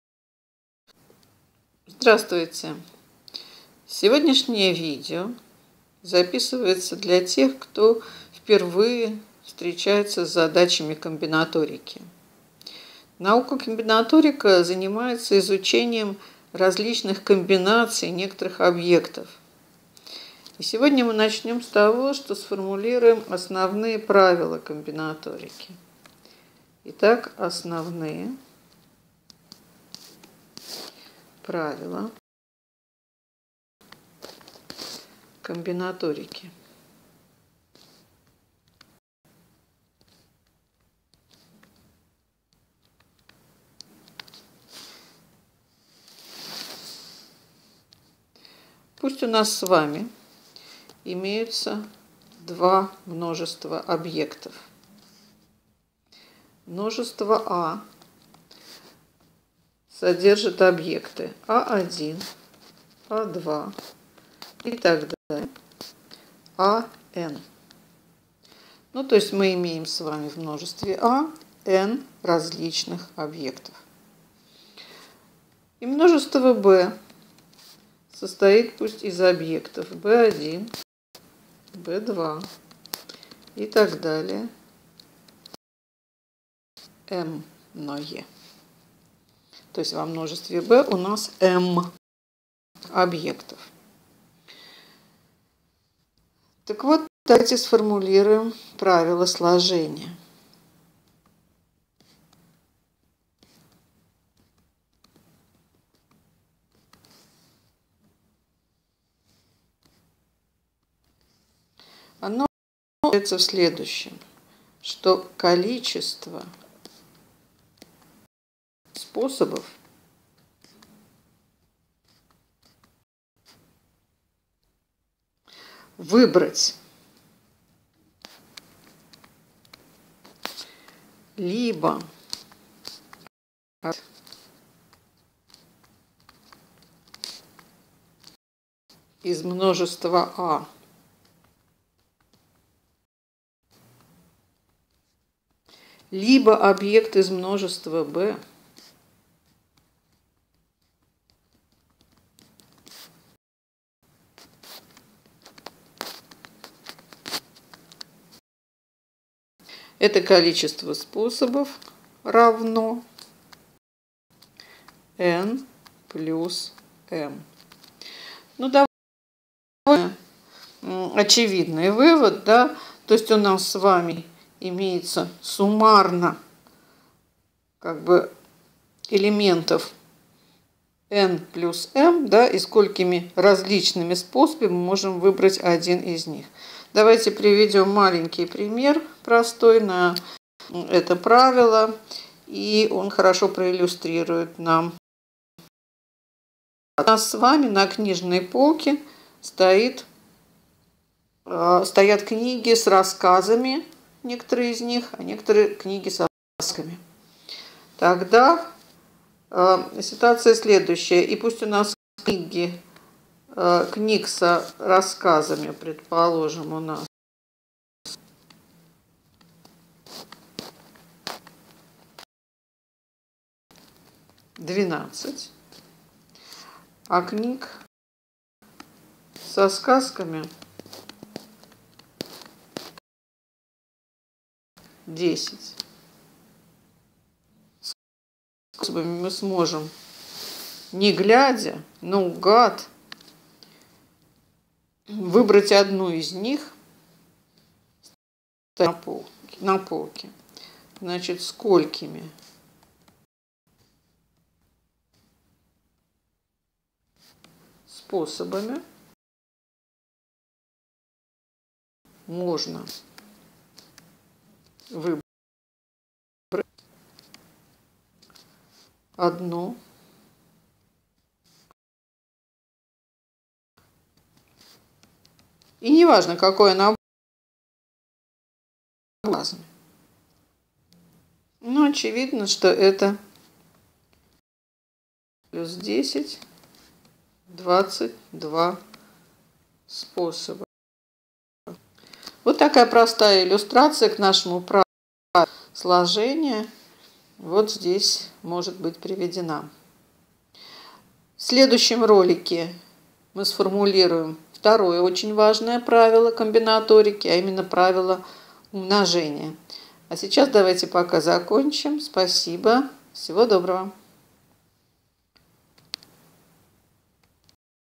Основные правила комбинаторики, правило сложения
Первое вводное видео плейлиста по комбинаторике. Сформулировано правило сложения, разобран простейший пример.